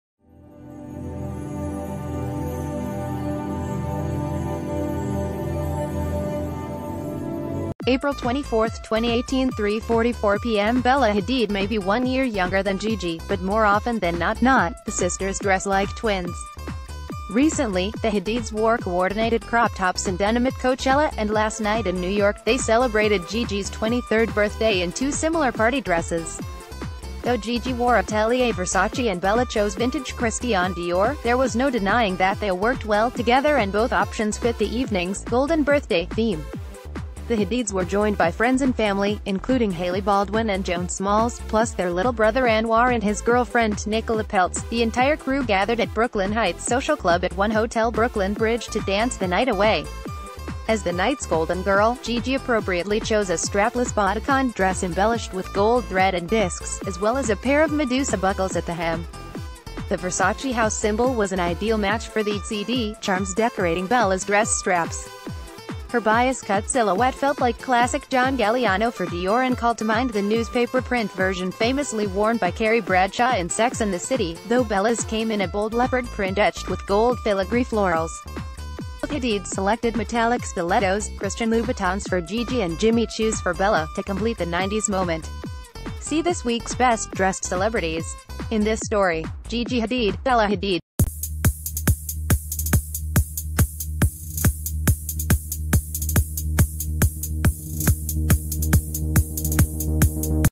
Breaking News | The Hadid Sisters Wear Matching Party Dresses on Gigi’s Golden Birthday
April 24, 2018 3:44 PMBella Hadid may be one year younger than Gigi, but more often than not not, the sisters dress like twins. Recently, the Hadids wore coordinated crop tops and denim at Coachella, and last night in New York, they celebrated Gigi’s 23rd birthday in two similar party dresses. Though Gigi wore Atelier Versace and Bella chose vintage Christian Dior, there was no denying that they worked well together and both options fit the evening’s “golden birthday” theme. The Hadids were join...